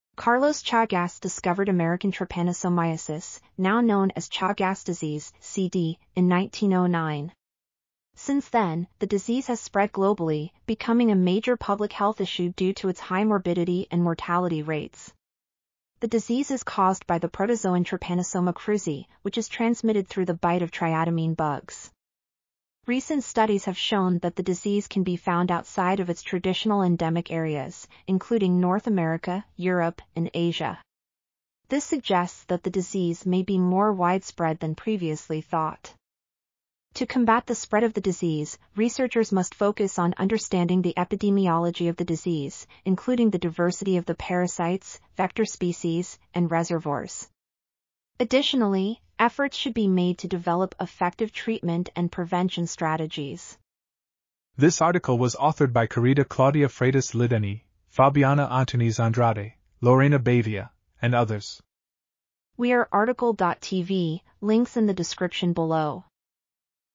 Chagas Disease: From Discovery to a Worldwide Health Problem | RTCL.TV
### Keywords ###

### Article Attribution ###
Title: Chagas Disease: From Discovery to a Worldwide Health Problem
Authors: Kárita Cláudia Freitas Lidani, Fabiana Antunes Andrade, Lorena Bavia, Flávia Silva Damasceno, Marcia Holsbach Beltrame, Iara J. Messias-Reason, Thaisa Lucas Sandri ,and Thaisa Lucas Sandri

### Video Timestamps ###
0:00:00 - Summary
0:00:54 - Title
0:01:02 - Outro
0:01:06 - End